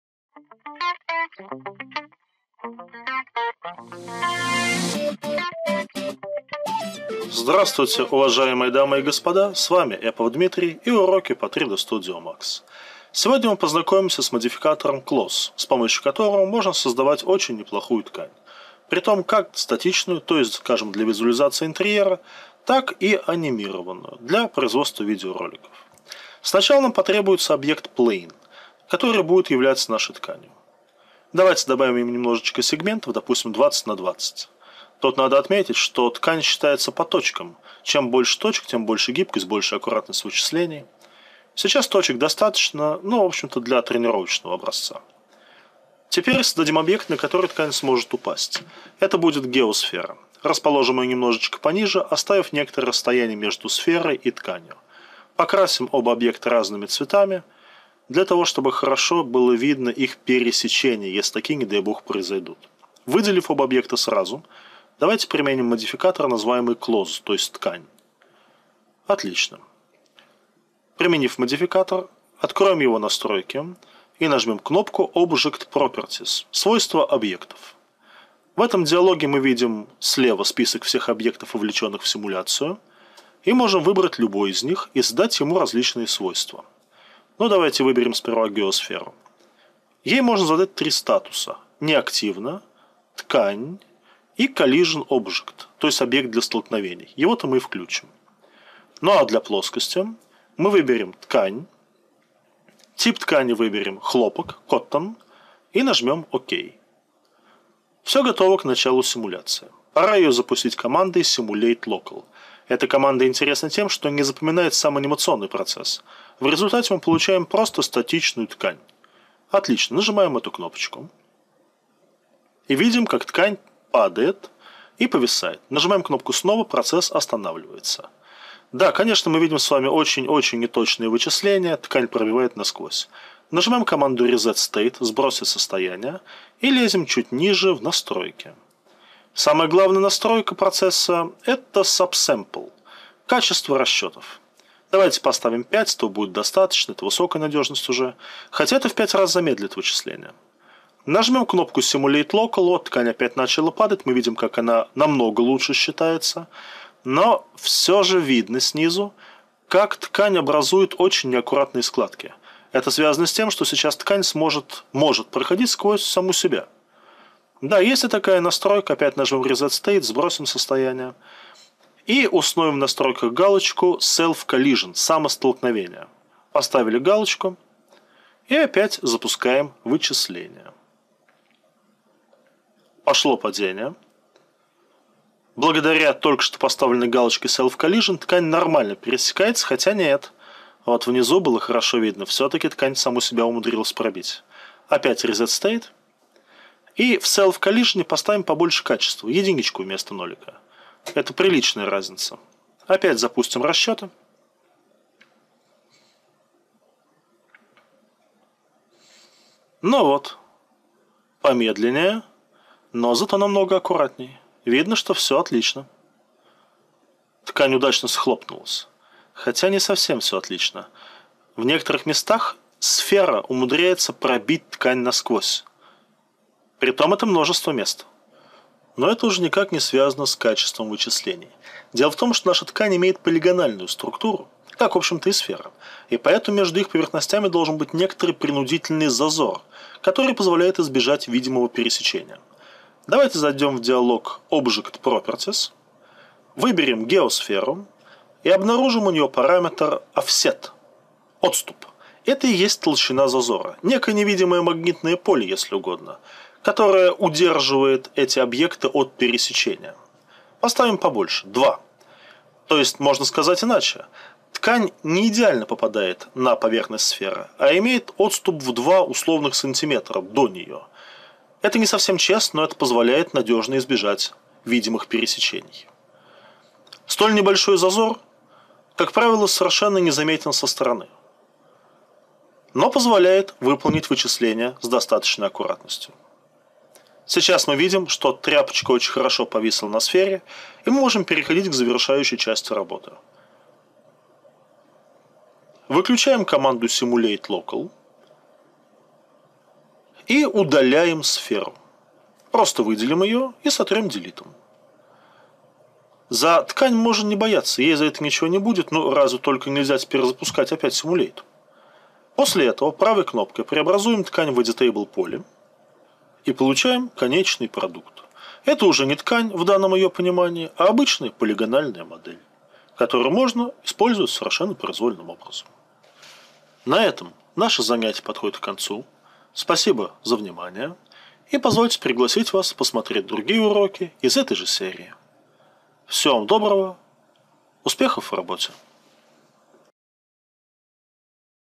3ds max - cloth 1 - основы.
Моделирование ткани в 3dsmax с помощью модификатора cloth, часть1 - основы.